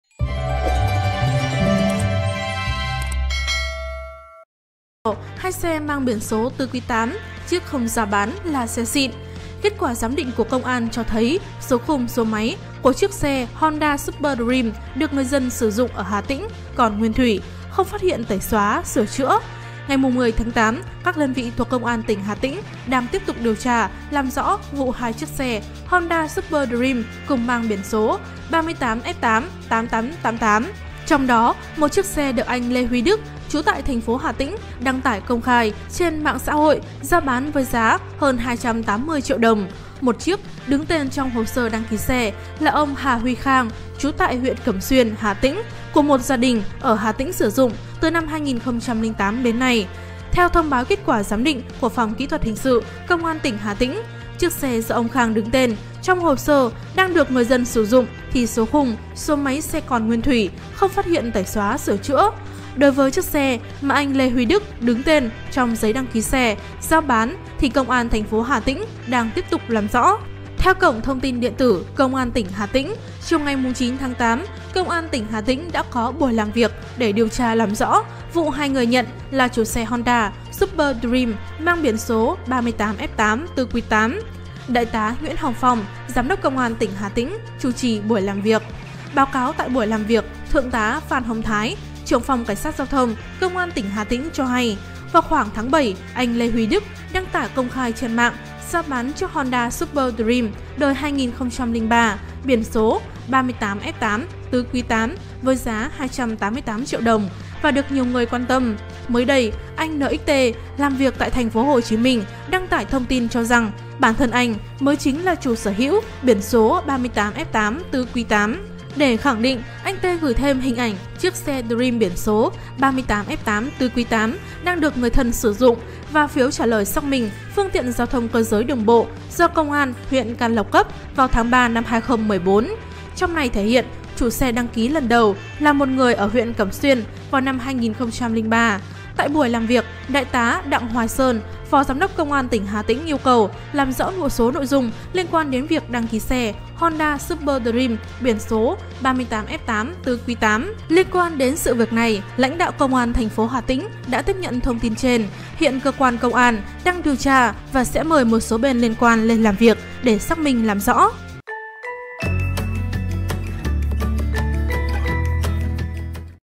2 xe mang biển số tứ quý 8888: Chiếc không rao bán là xe “xịn”
Kết quả giám định của công an cho thấy số khung, số máy của chiếc xe Honda Super Dream được người dân sử dụng ở Hà Tĩnh còn nguyên thủy, “không phát hiện tẩy xóa, sửa chữa”